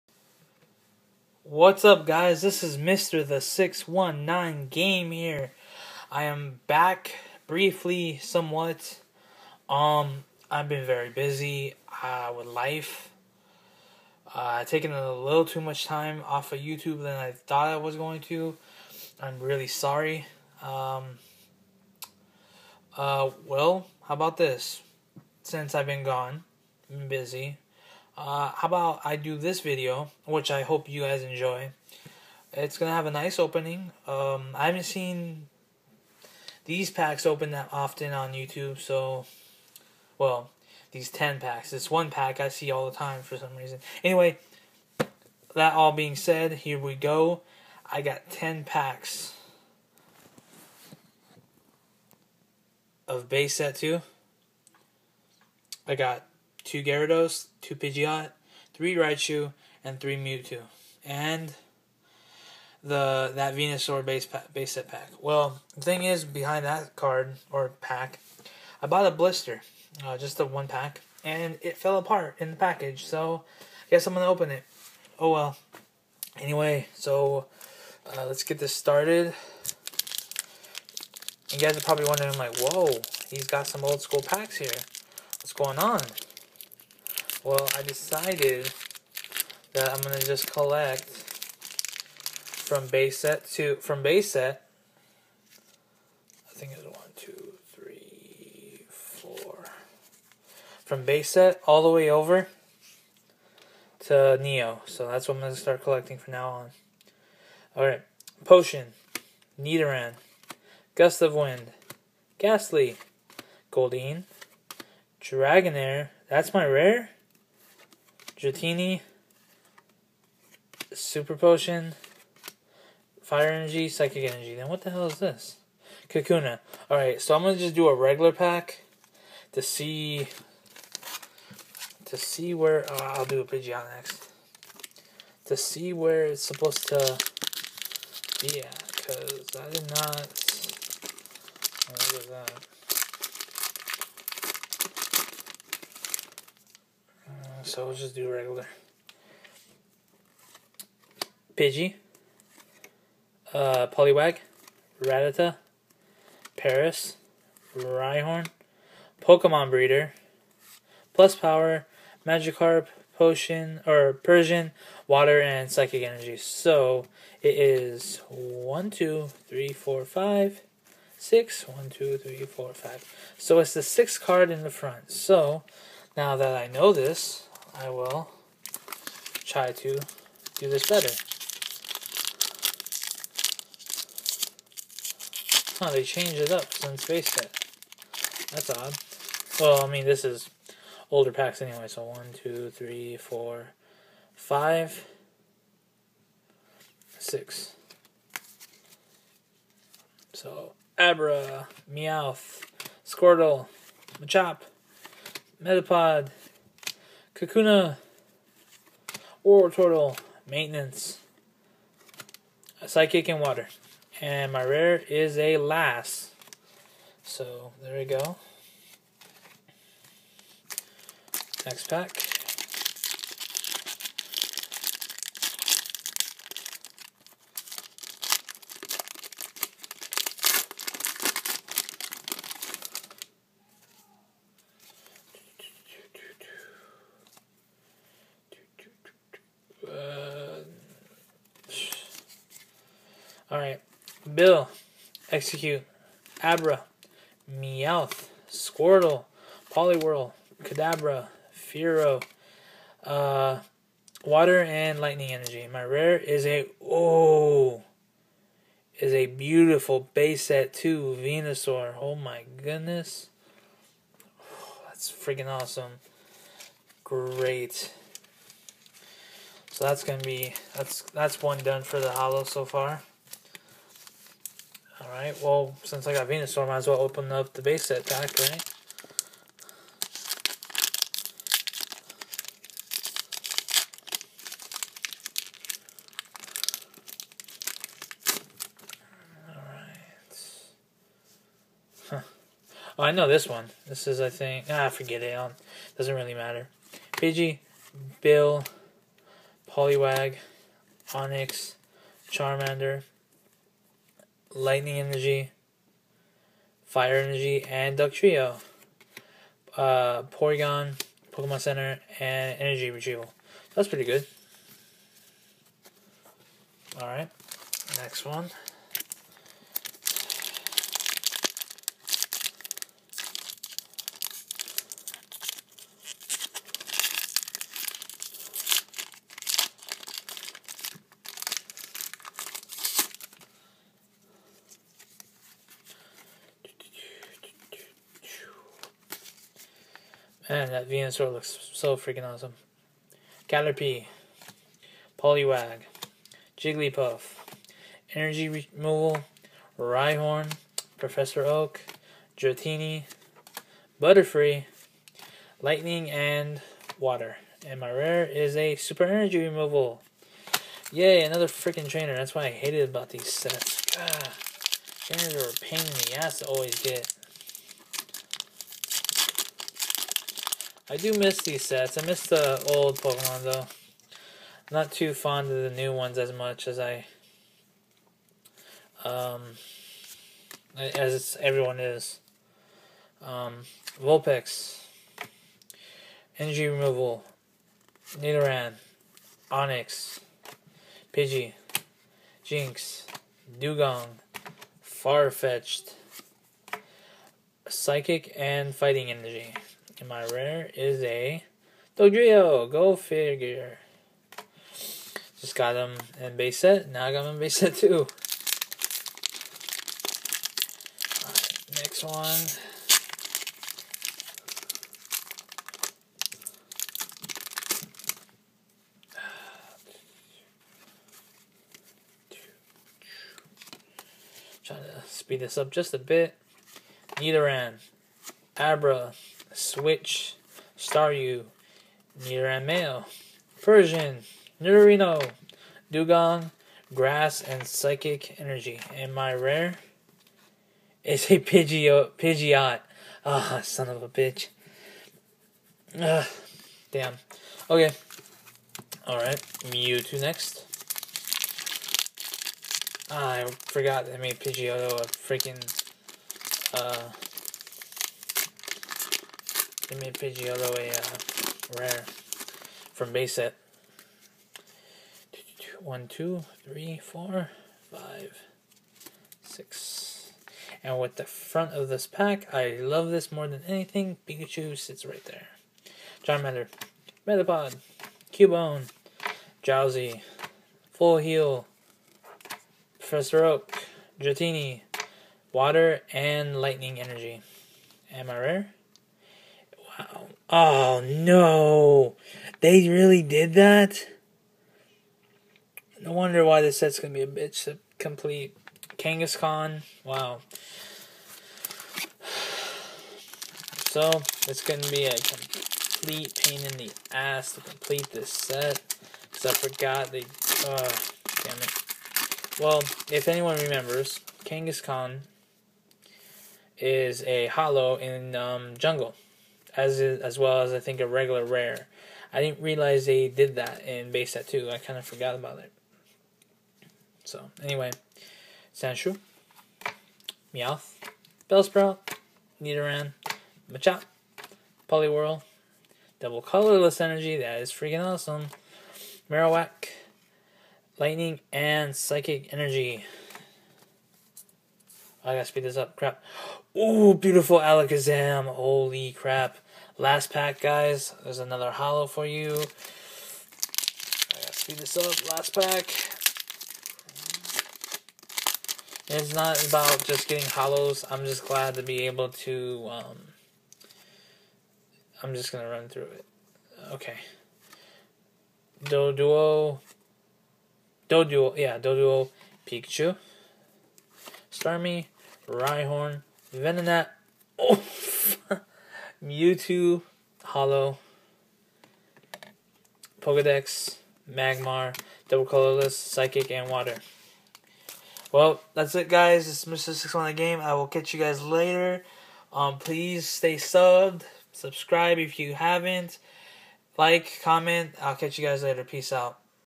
MrTHE619GAME Old School Pack Opening !!
Here's some packs of base set 2 that I got on eBay for a good price. I am trying to come back into poketubing. Hope you enjoy !!